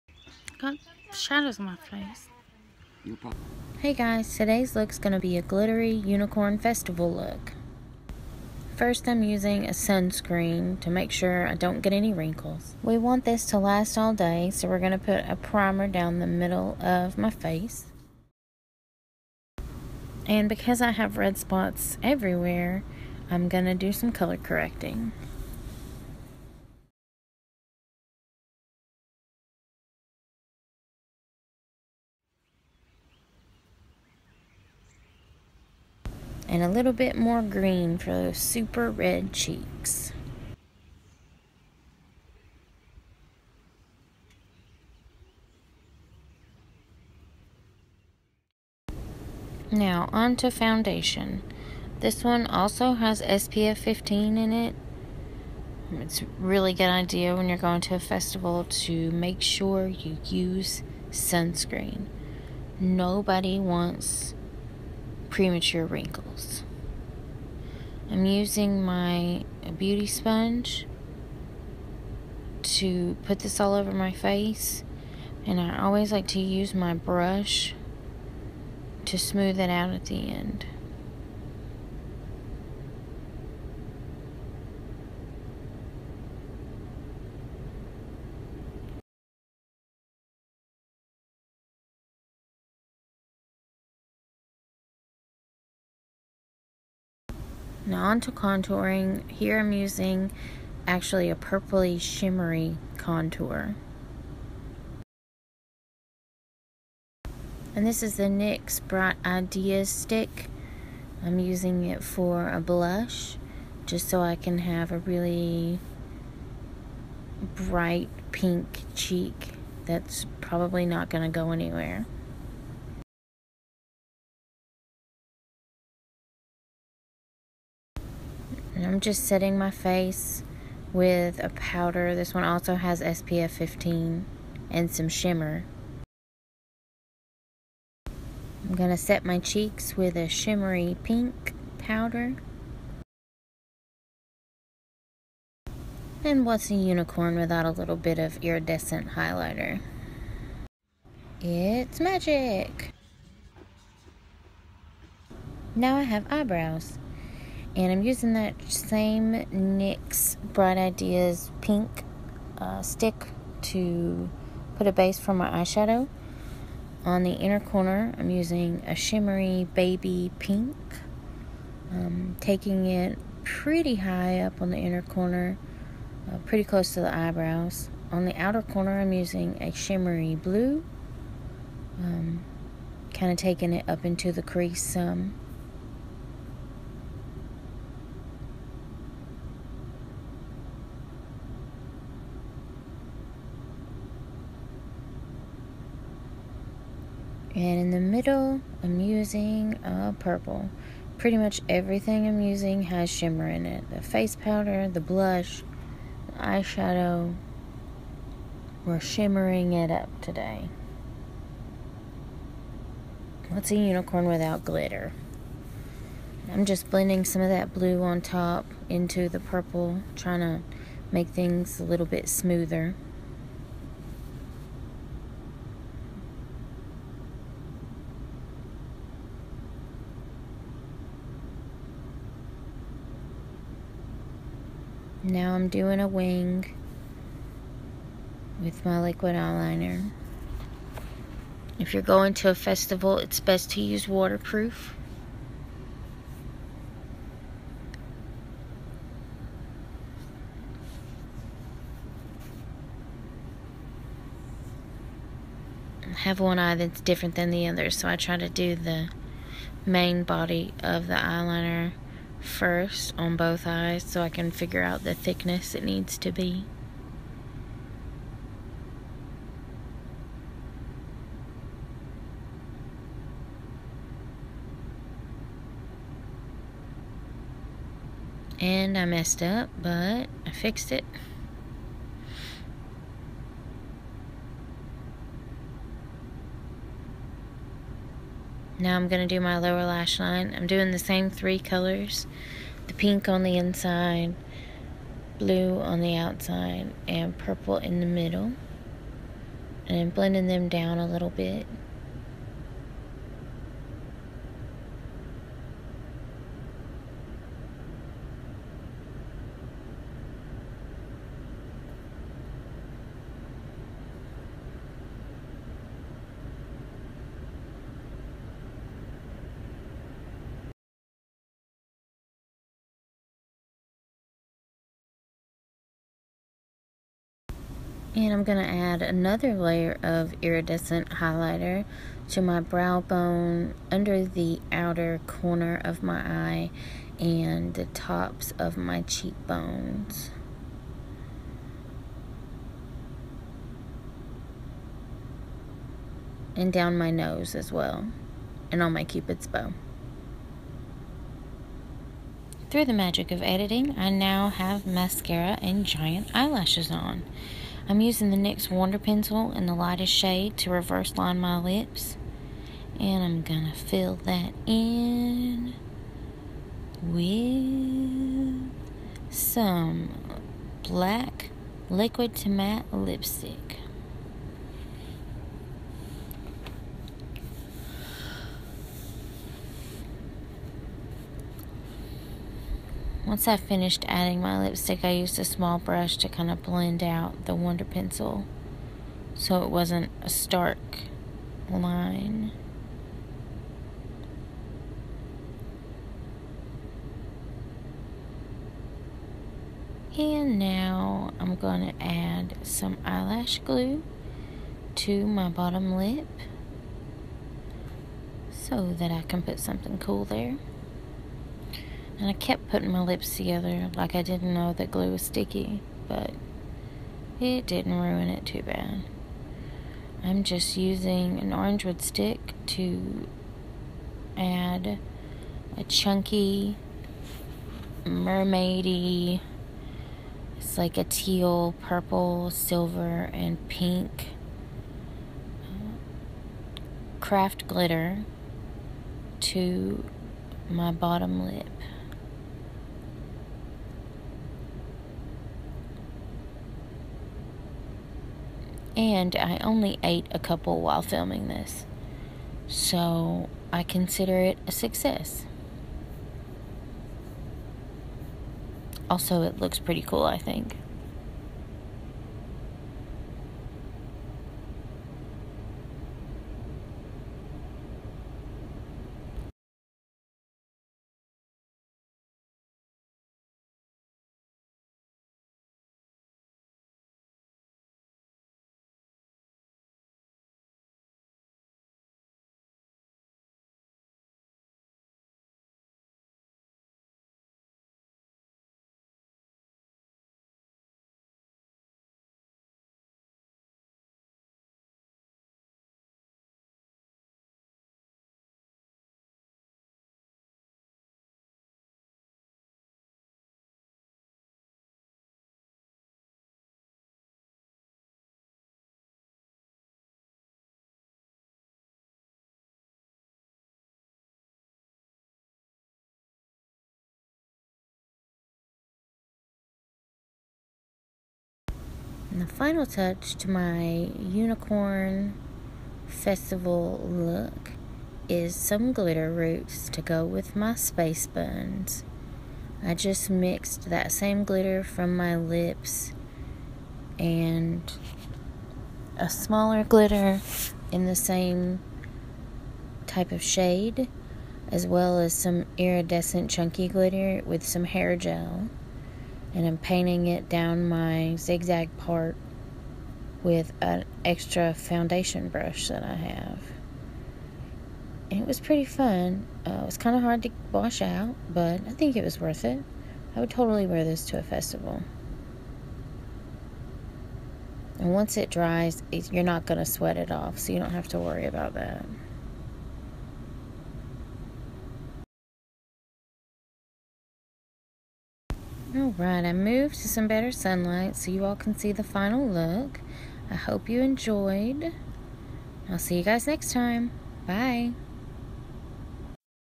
NYX Face Awards 2017 USA Entry Glitter Unicorn Festival face
Hey guys! This is my first entry to the NYX Face Awards. I had so much fun making this glittery Unicorn inspired festival look!! I just wish I had somewhere awesome enough to wear it.